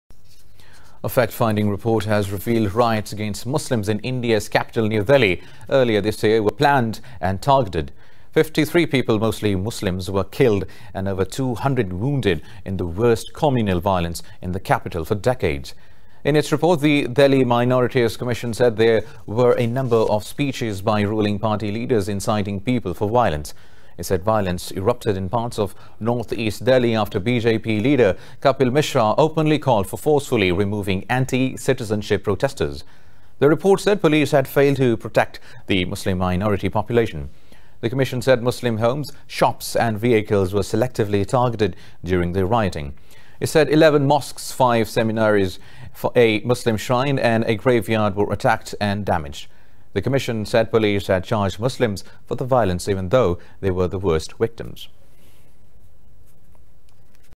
India: Report Slams Police Role In Riots | Indus News | Indus News Bulletin
A fact-finding report has revealed, riots against Muslims in India’s capital NewDelhi earlier this year were planned and targeted.